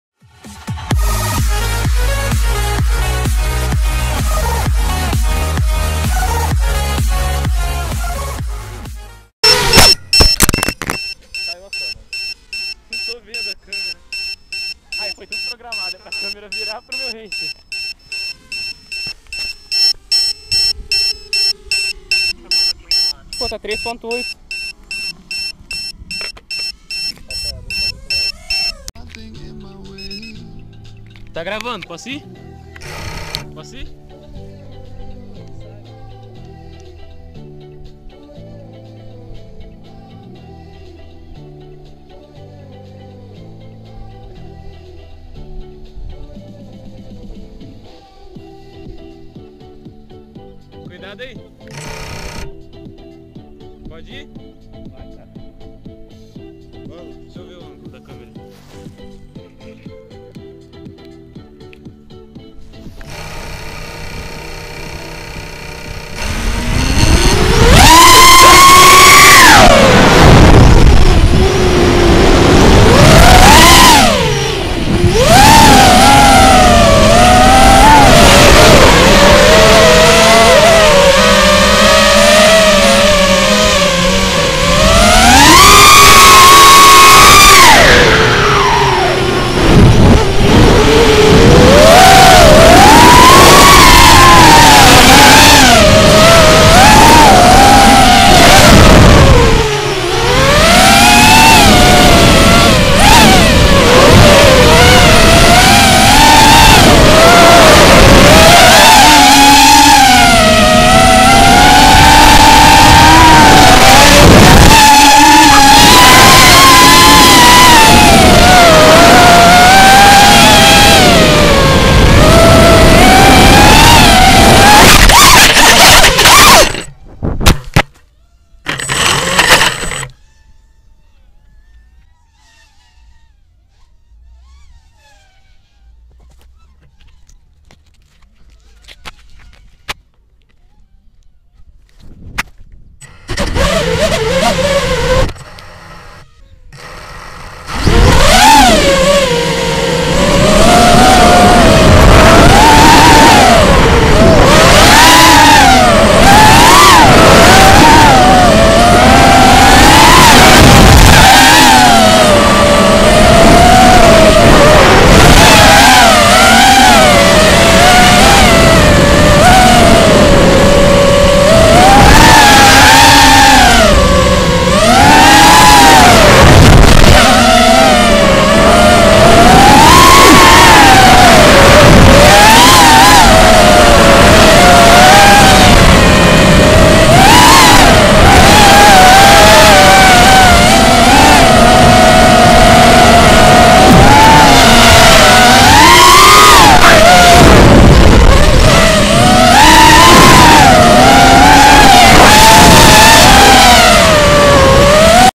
best baturay of day 16/10/2016 (raw seres)
Pilot: Lukas Voulgarelis (Suicide Quad )

Location: Brazil, Rio de Janeiro, Sao Pedro da Aldeia, Base aeronaval de Sao Pedro da Aldeia, GAM (Gremio aeronaval de modelismo)

Setup:
VTX: immersion 600mw
Propeller: DAL 5x4 triblade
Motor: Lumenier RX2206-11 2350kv
ESC: Kiss 24A
FC: Kiss fc
FPV cam: HS1177 (original lens)
FPV antenna: BeeRotor
Frame: QAV-R 5'
Battery: Lumenier 1300mah, 60-120c, 4s
Cam: Xiaomi Yi (1080p, 60fps)